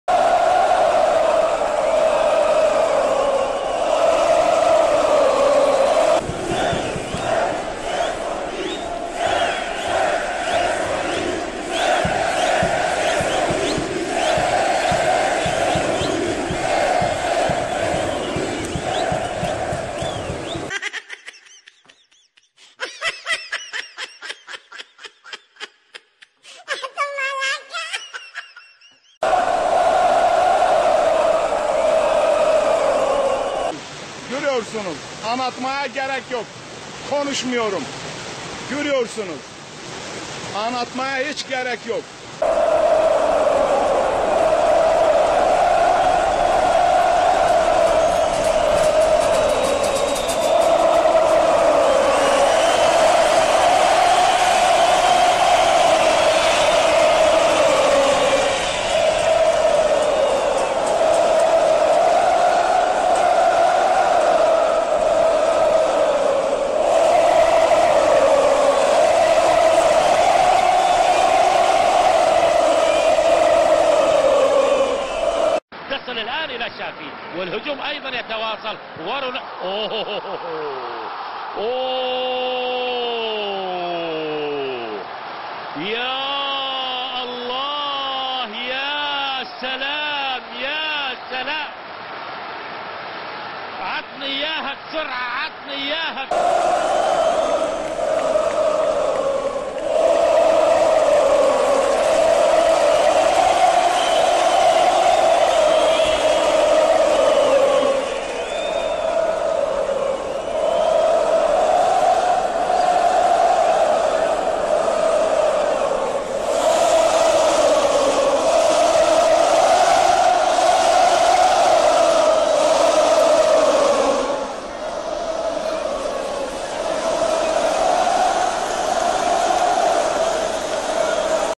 Halı Saha Maçından Eğlenceli Anlar :)
thermonox - bse halı saha maçından komik ve eğlenceli anları sizin için derledim. umarım begenirsiniz. :)))